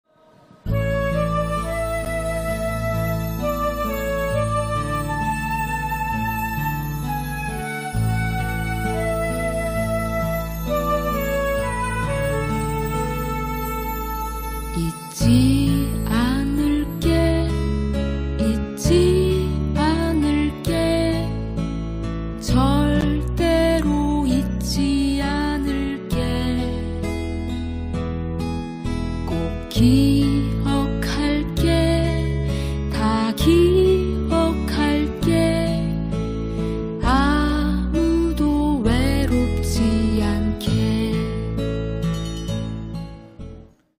10.29 이태원 참사 희생자 추모 묵념 '김건희특검·윤석열퇴진 18차 촛불대행진'(2022.12.10.)
김건희특검·윤석열퇴진 18차 촛불대행진

2022년 12월 10일 오후 4시 숭례문 앞